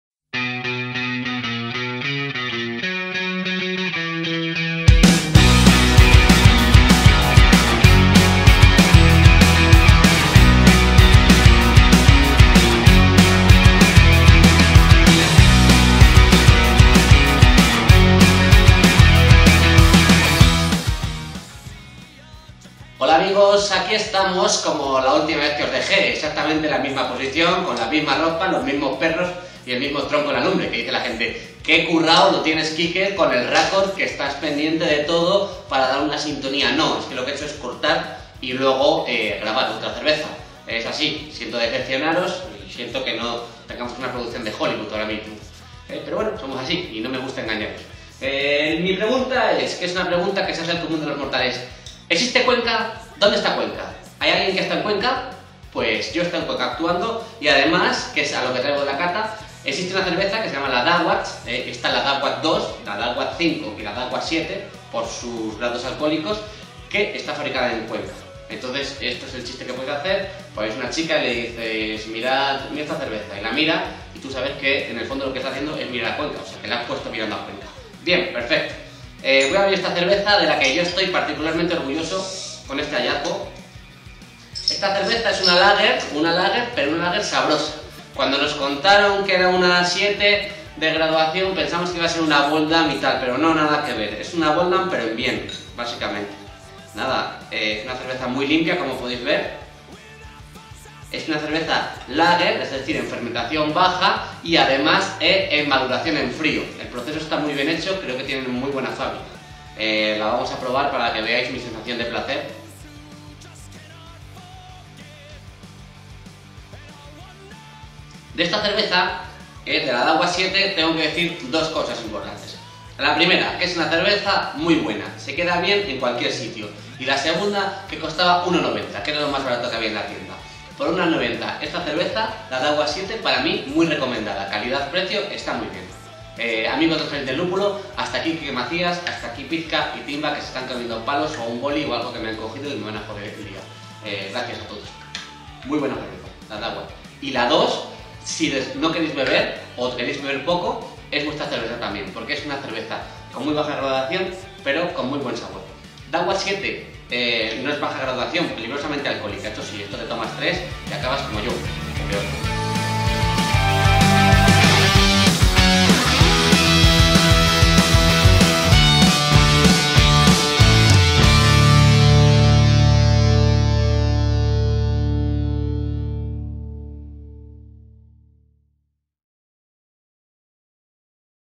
Vídeo-cata: Quique Macías y la Dawat 7
Quique Macías bebe y critica para el blog especializado en cerveza "El Jardín del Lúpulo" la cerveza española de baja fermentación Dawat 7 (hecha en Cuenca).

O de comentar abajo qué os ha parecido.